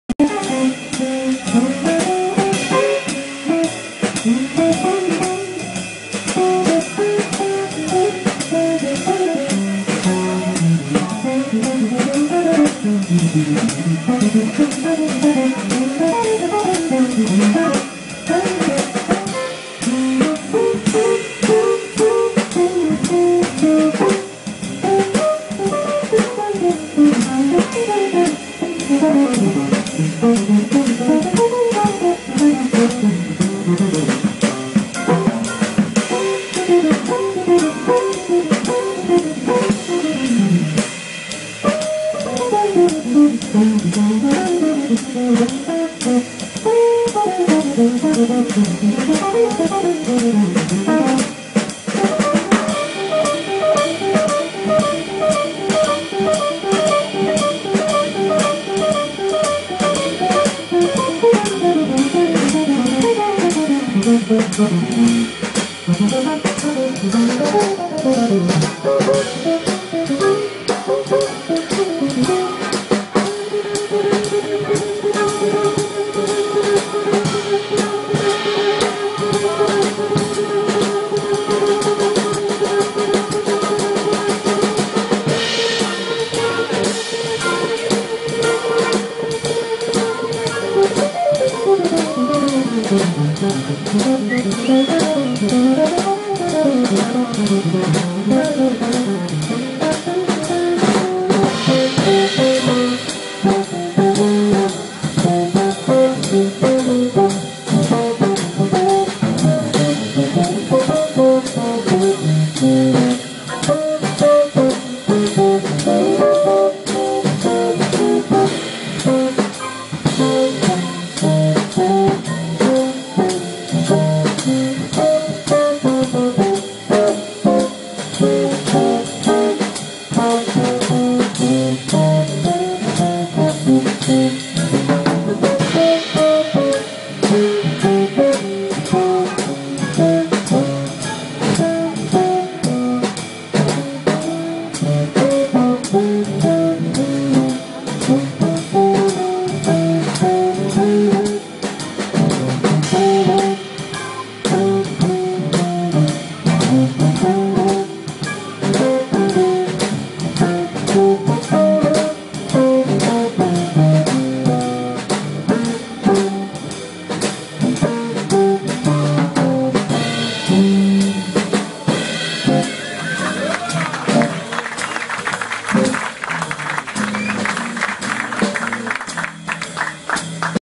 Pat Martino at Jimmy Glass Jazz Festival
III Festival Int. Jazz Contemporáneo del JIMMY GLASS
PAT MARTINO TRIO (17/11/2013)
PAT MARTINO, guitarra
PAT BIANCHI, órgano
CARMEN INTORRE JR, batería
Jimmy Glass Jazz since 1991 (Valencia, España/ Spain)